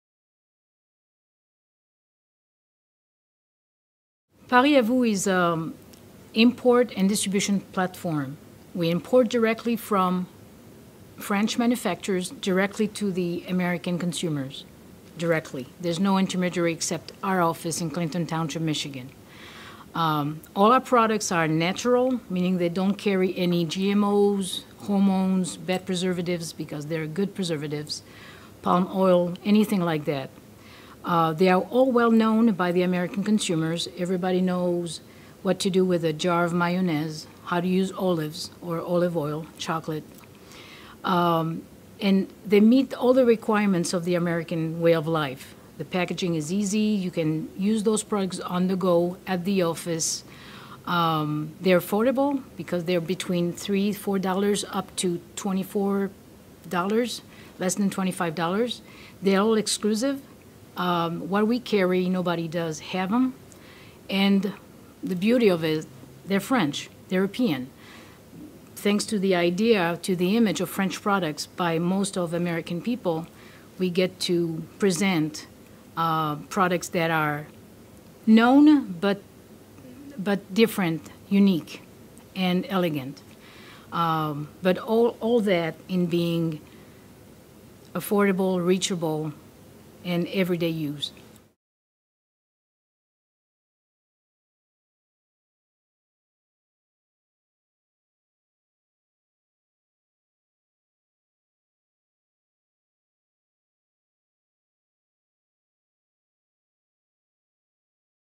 Paris à Vous and its Products: Interview with Geraldine Portelli
Paris a Vous sells natural, well-known, exclusive, affordable, european products that meet the requirements of the american way of life.

Founder and CEO, Geraldine Portelli, explains why Paris a Vous' products are so sought after.